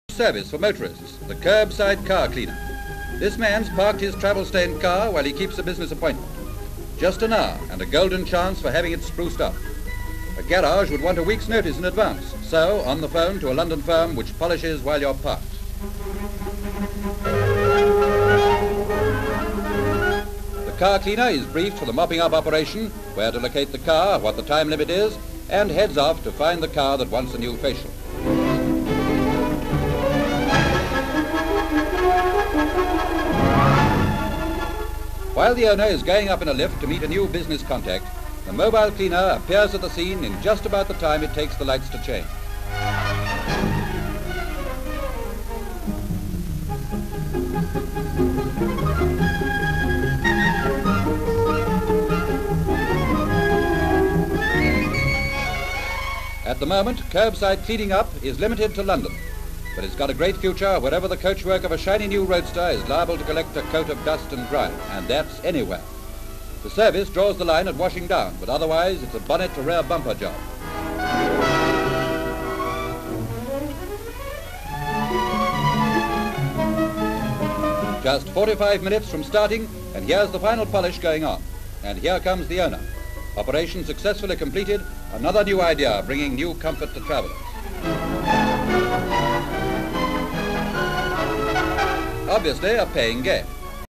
Kerbside Car Cleaners (1949)
Kerbside car cleaners.

Buckingham Palace Gardens, London.

L/S of a man getting out of his car and entering a telephone box. C/U as he dials the number for the "Kerbside Kar Kleeners" run by Colonel Lawson, M/S looking in from outside. M/S of the garage man at a window where he receives his instructions. He gets on his motorcycle and rides to where the man's car is parked. Various shots as he cleans and polishes the car.  C/U of a chauffeur watching. C/U as man continues to clean the car. L/S as the owner returns and looks over the car.
 FILM ID:1265.29